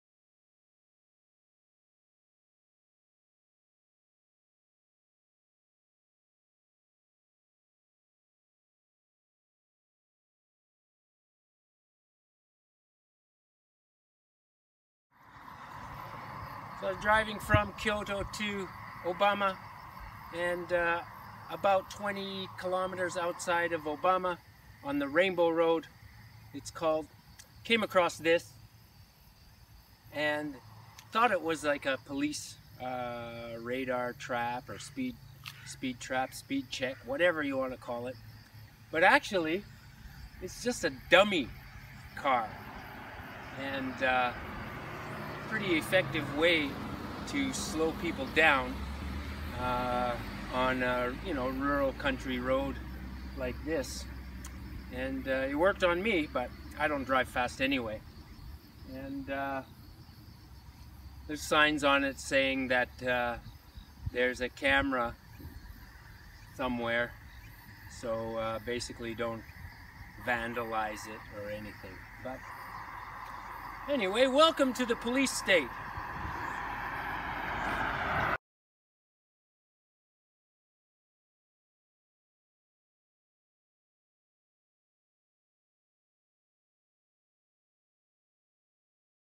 Dummy Police Cars Slow Down Motorists 🚓
I come across an effective way to slow down motorists in Japan.

If you enjoyed and got value from this video, please help my channel grow.

Bitcoin:
3PsUQnVnoqwsRXR1aj78vU4CXt2ae99cZd